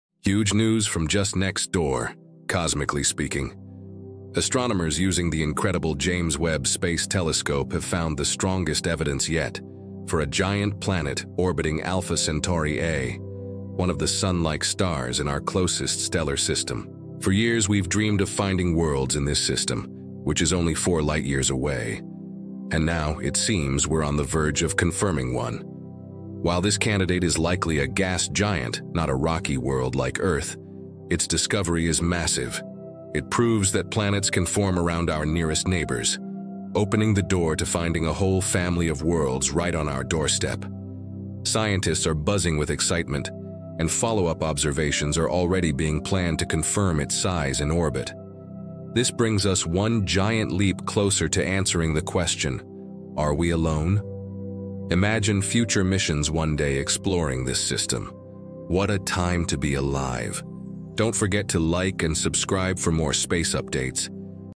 New Evidence of a Giant Planet Orbiting the Closest Sun-Like Star!
🚀 Breaking News from JWST: Astronomers have found strong evidence of a giant planet orbiting Alpha Centauri A — the star nearest to our Sun. While not yet a rocky world, this discovery could mark the closest exoplanet around a Sun-like star ever detected. Learn why this matters, what comes next, and how it changes our view of the universe.